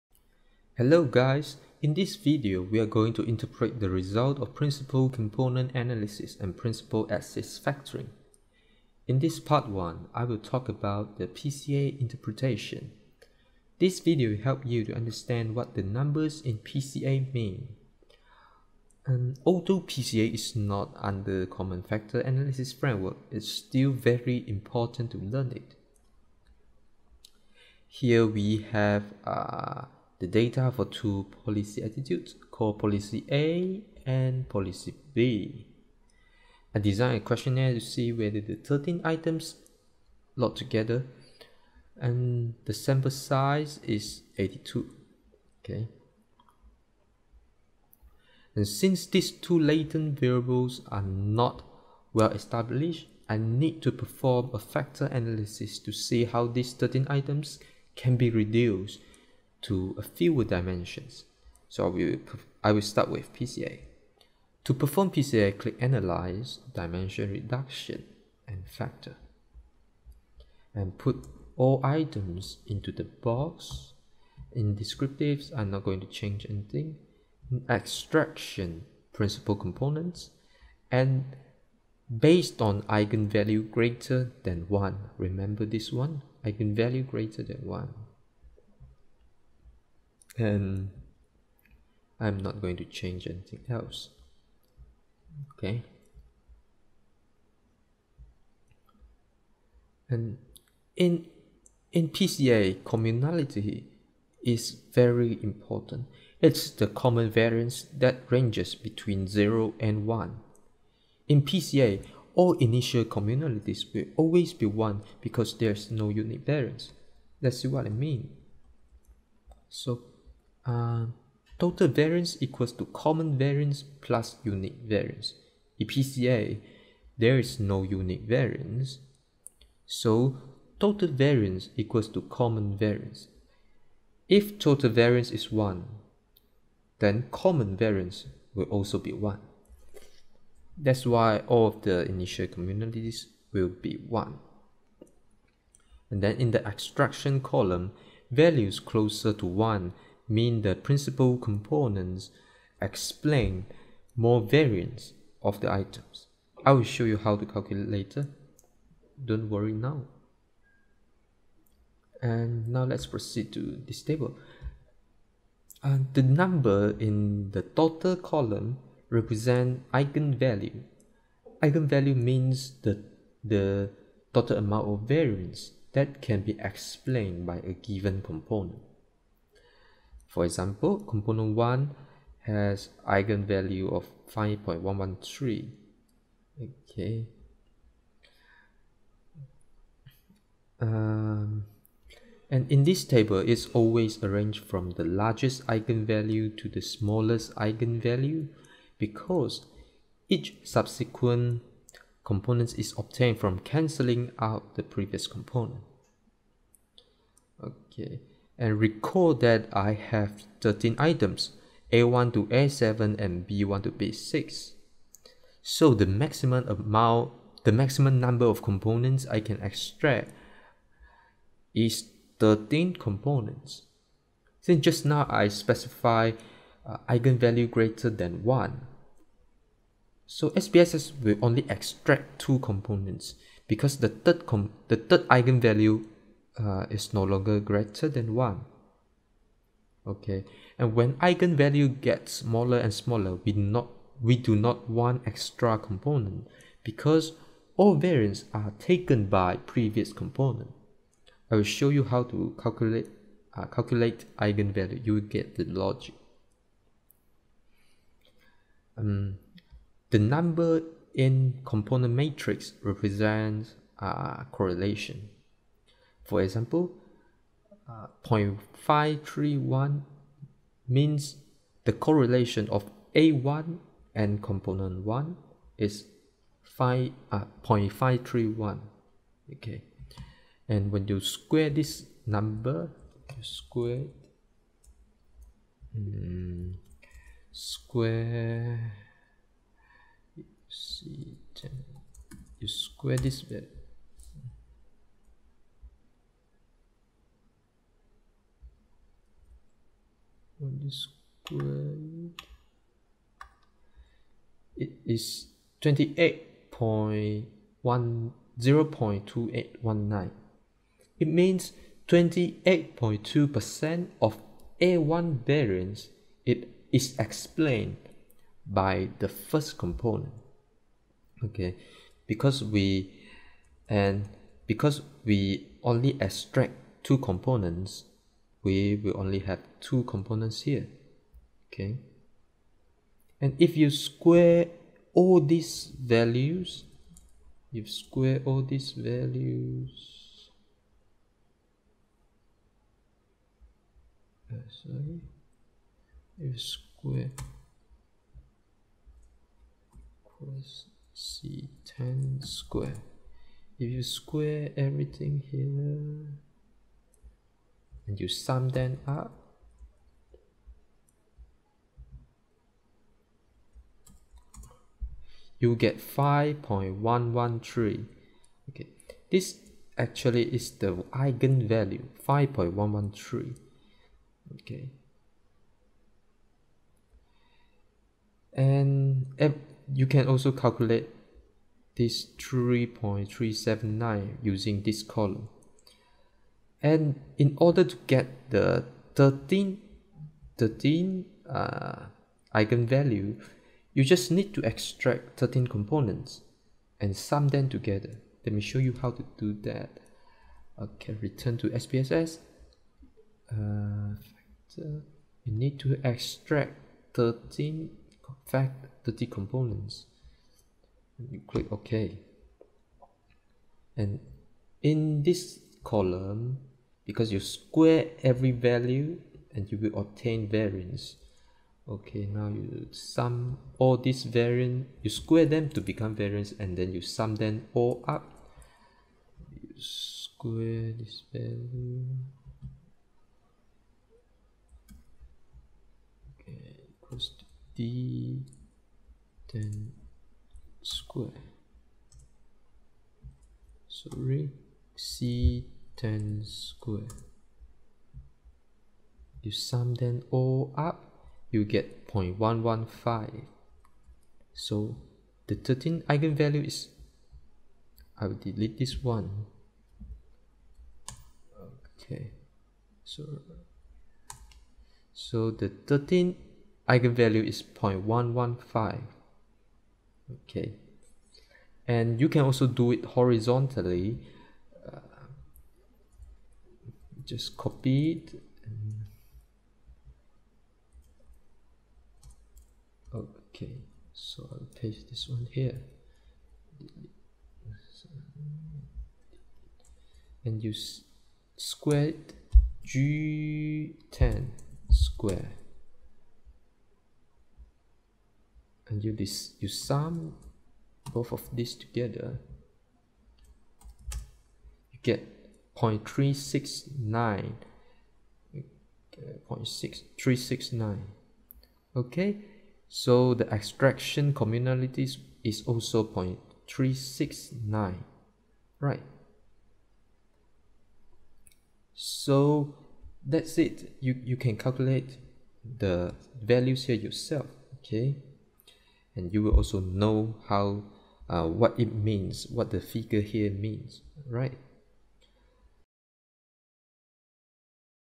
Communality & Eigenvalue - Calculation and Interpretation, PCA SPSS
This video shows the calculation and interpretation of communality and eigenvalue in PCA, SPSS. It provides relevant explanations to viewers. You are encouraged to pause the video as the video includes written explanations that you may want to read. Hope it is helpful to you guys.